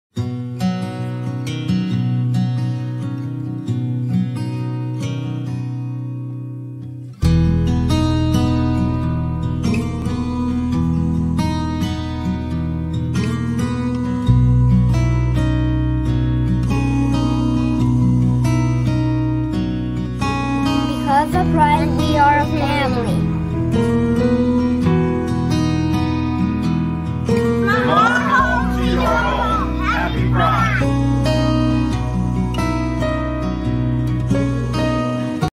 Happy Pride Fresno 2020 "Gay Dads and their kids"
What PRIDE means to us as gay dads raising 2 little boys.